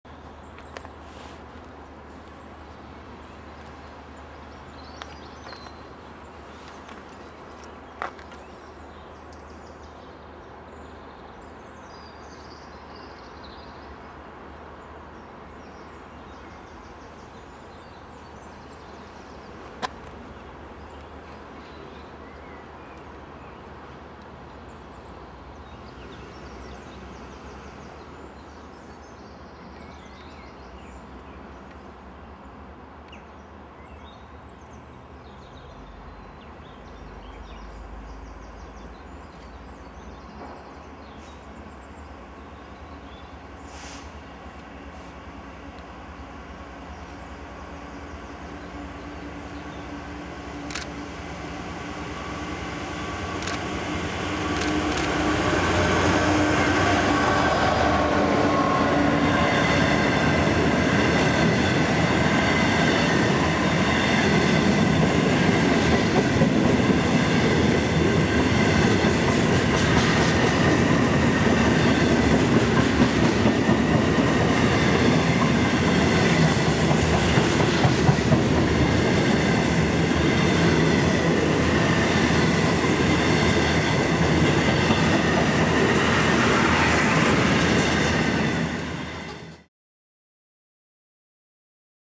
66024 on 4H05, 04:30, Ratcliffe Power Station - Daw Mill
66024 on 4H05, 04:30, Ratcliffe Power Station - Daw Mill empty HTAs
Water Orton
29 May 2008